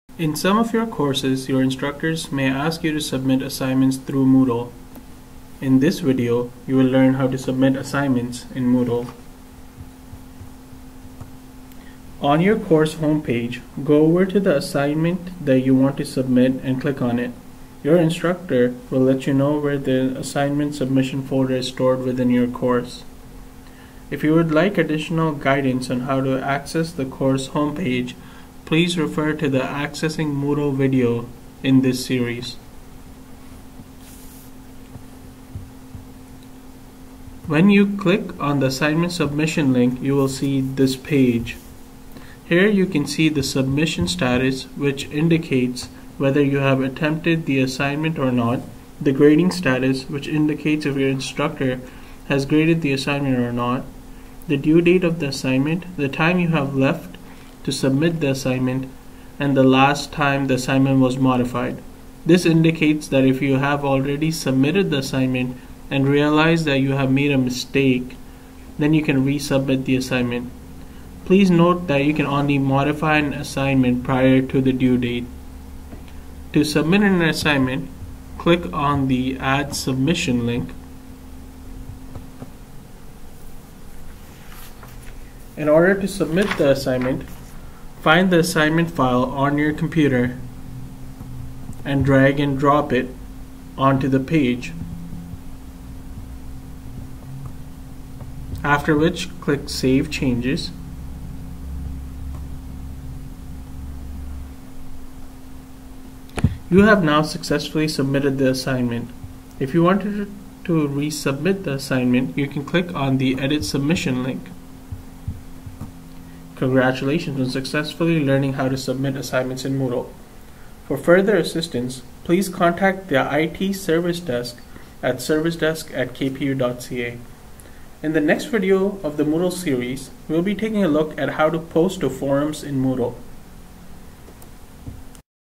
Submitting Assignments in Moodle
In this video, we will take a look at how to submit assignments in your Moodle course.